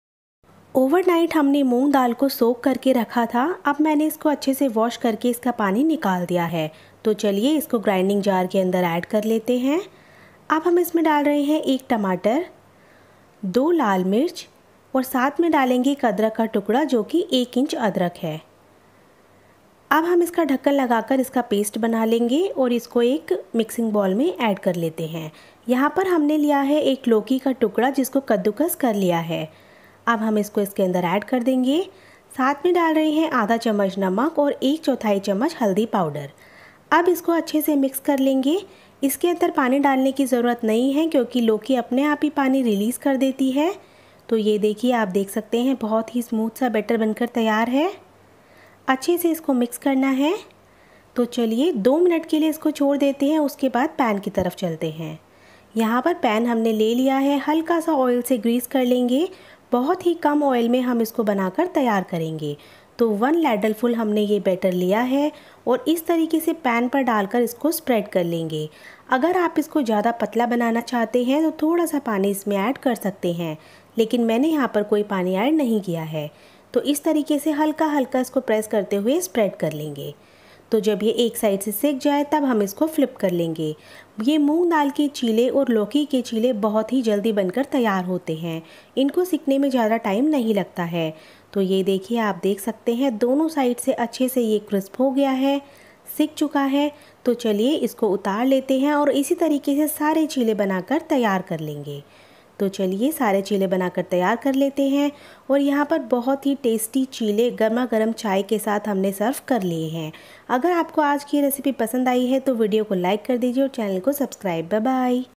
सबसे आसान Quick And Healthy Morning Breakfast Recipe | Breakfast Recipe In Hindi
today in this video I am sharing a very healthy morning breakfast recipe, it's a less oil recipe which can be enjoyed by a diabetic also...